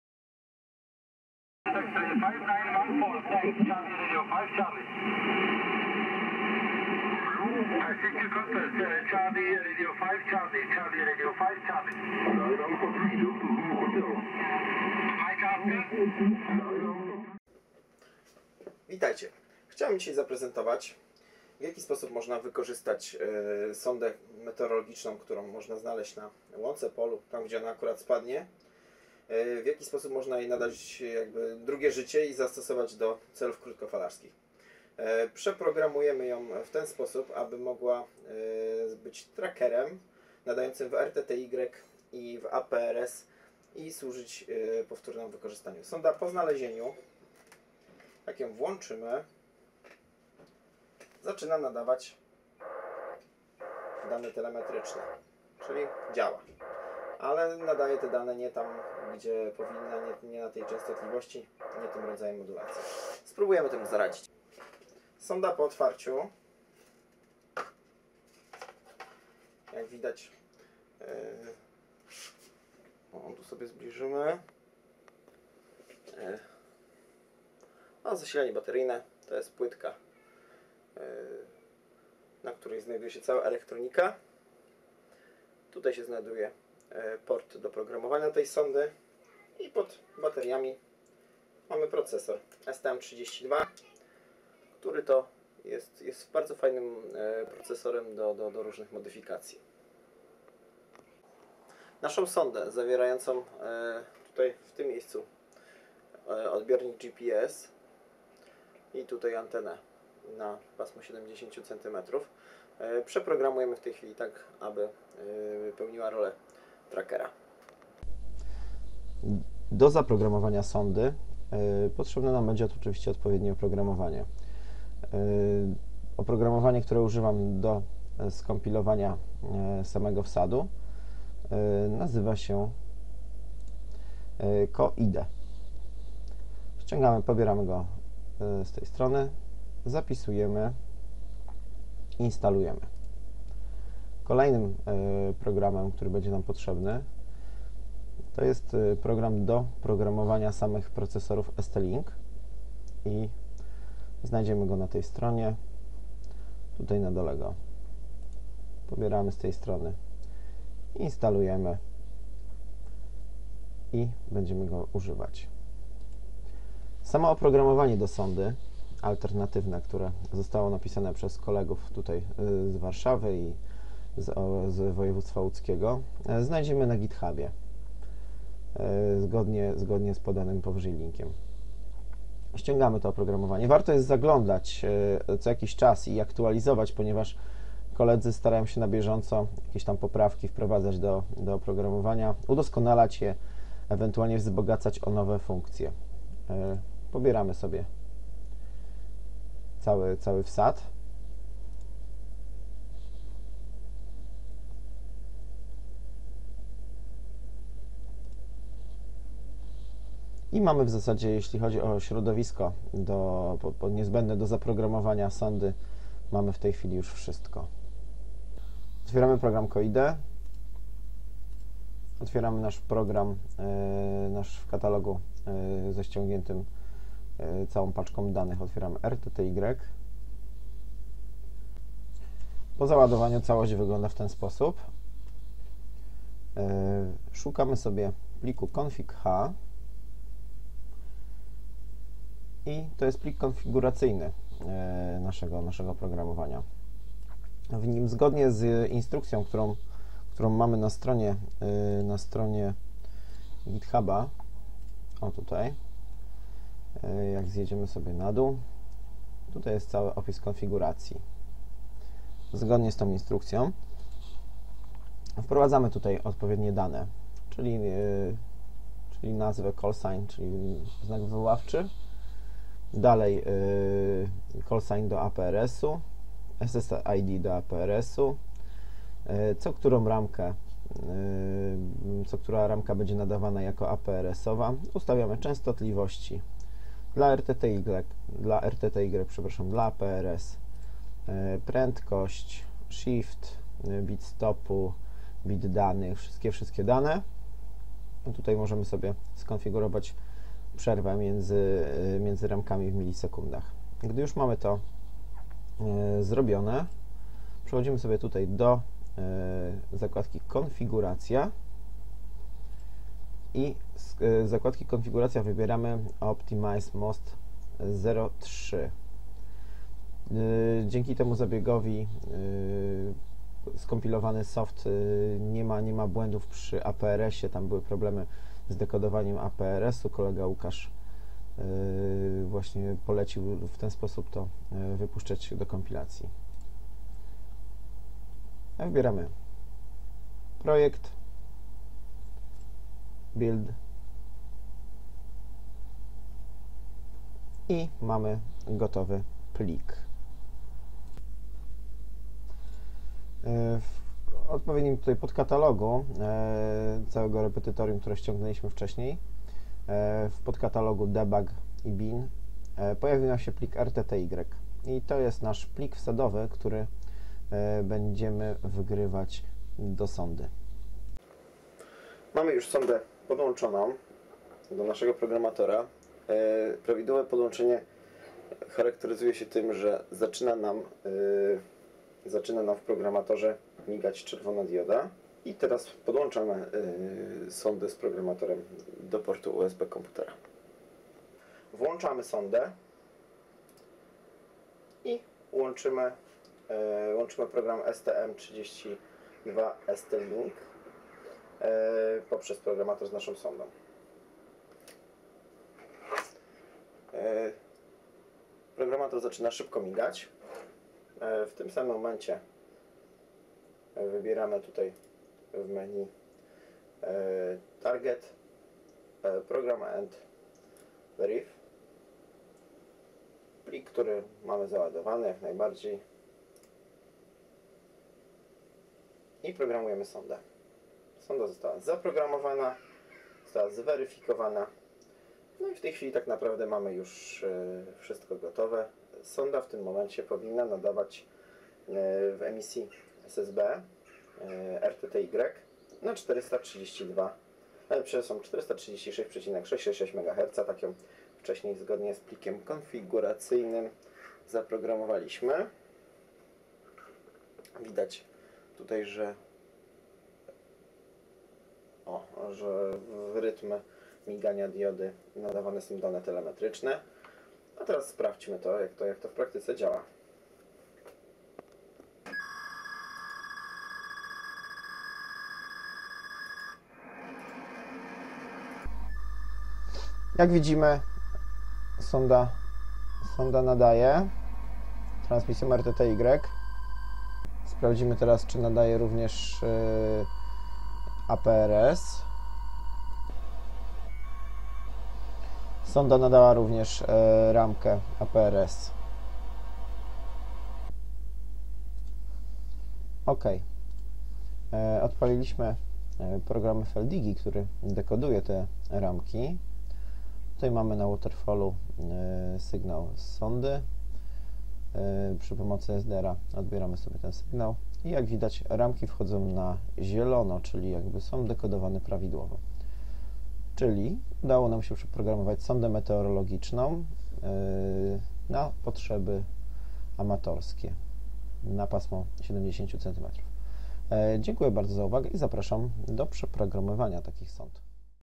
Sonda meteo RS41 nadająca w RTTY i APRS
Opis przeprogramowania sondy meteo RS-41 przy pomocy bezpłatnego i ogólnodostępnego w sieci oprogramowania.
Autorem oprogramowania są: SQ7FJB, SQ5RWU i SQ9MDD.
Tego typu eksperymenty wymagają posiadania uprawnień do nadawania w paśmie 70cm, a więc przeznaczone są dla licencjonowanych nadawców.
Dla pozostałych zainteresowanych sonda może nadawać w paśmie ISM czyli od 433,050 do 434,790 MHz